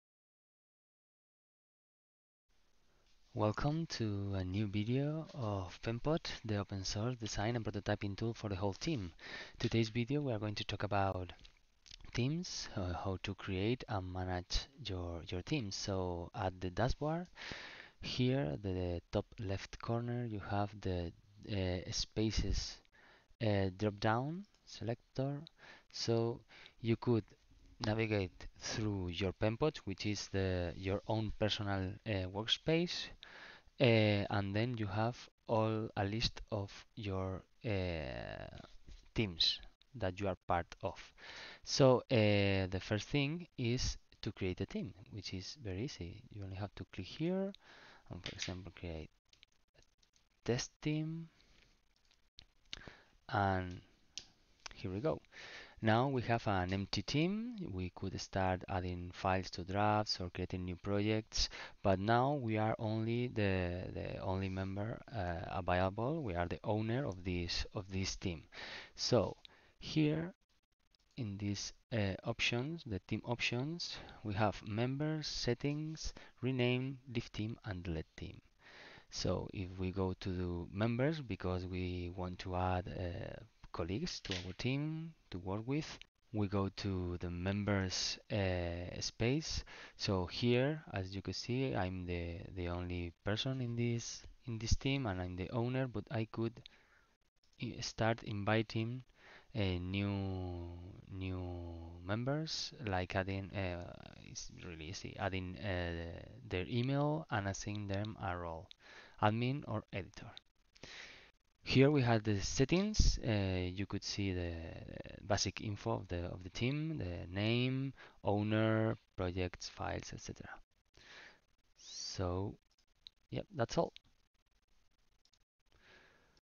Manage a team - Penpot fundamentals
This tutorial is part of the Penpot Fundamentals series.

Penpot is a free, Open Source and collaborative platform for design and prototyping. SVG & low-code. Design Freedom for Teams

Penpot: Design Freedom For Teams.